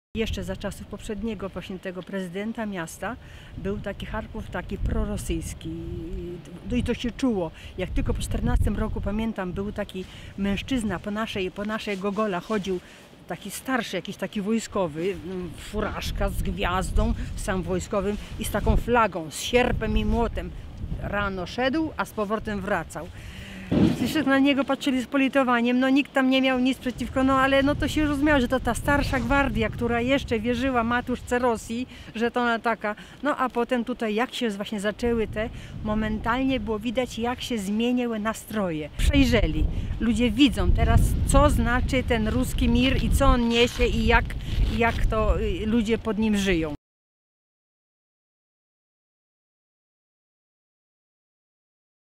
Charków. Kiedyś popierano tu Rosję, dziś zrozumiano, co znaczy "ruski mir"
Nasz reporter Paweł Kurek był w Charkowie, gdzie porozmawiał z polską zakonnicą, Sabiną. Dowiedział się, jak wyglądają nastroje w niegdyś prorosyjskim mieście.